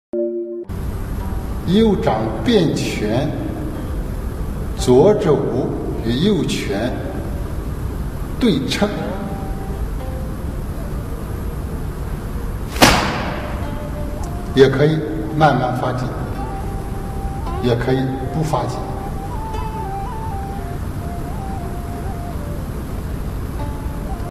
Chen style 9 form Tai Chi tutorial 8/9 Cover Hands and Strike with Fist (Right)
陈小旺九太极8右掩手肱拳